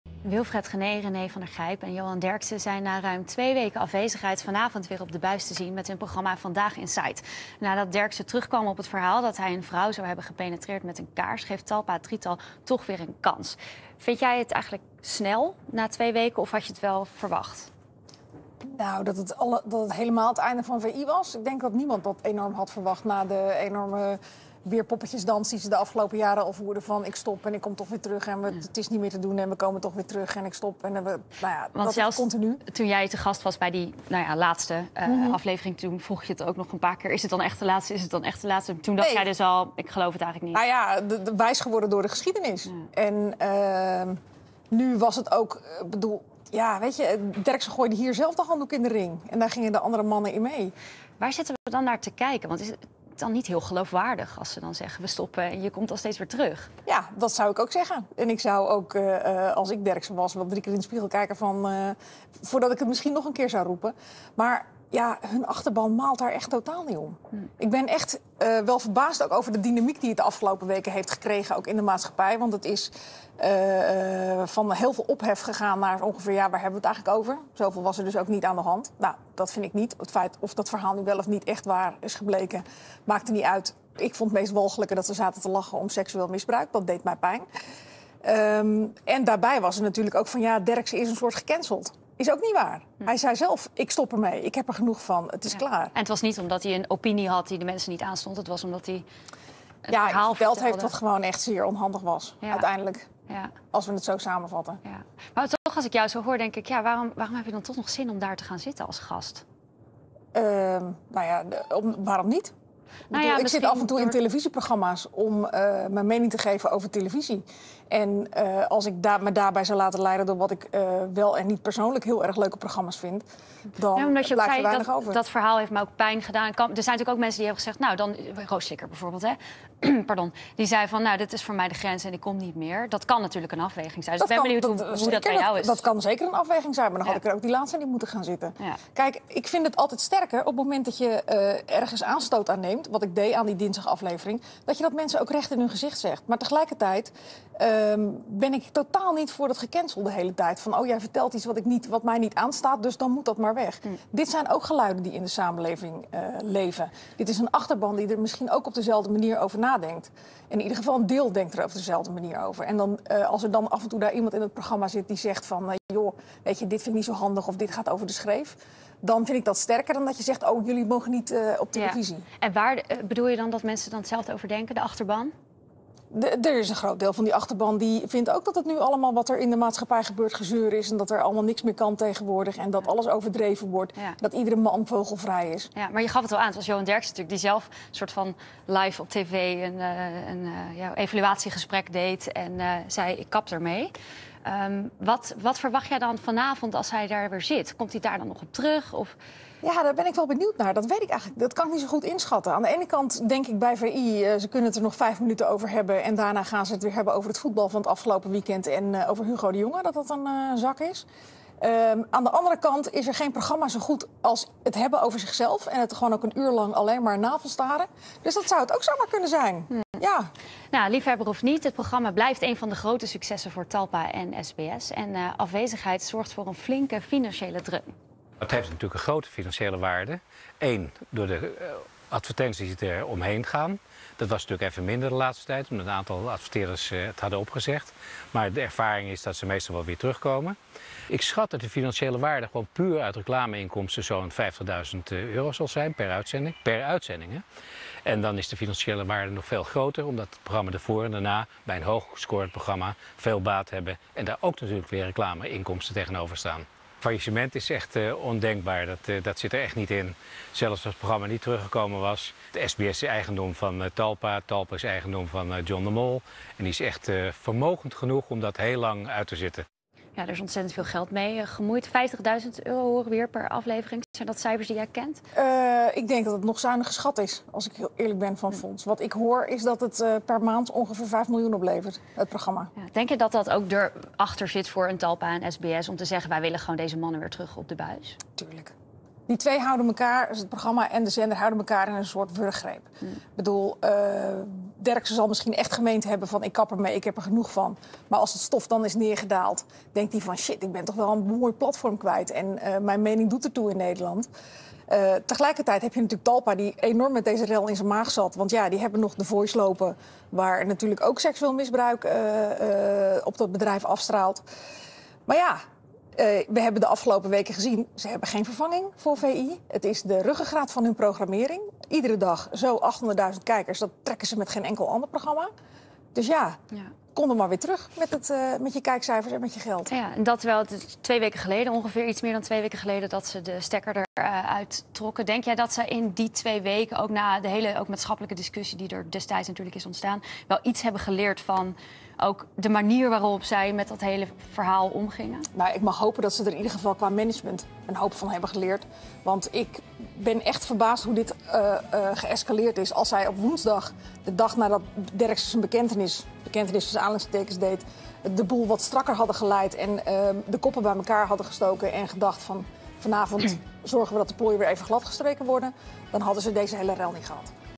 Speelt geld bij Talpa een rol om Vandaag Inside weer uit te zenden?
Vandaag Inside keert vanavond na een korte periode van afwezigheid weer terug op televisie. Wilfred Genee, René van der Gijp en Johan Derksen krijgen van Talpa opnieuw een kans.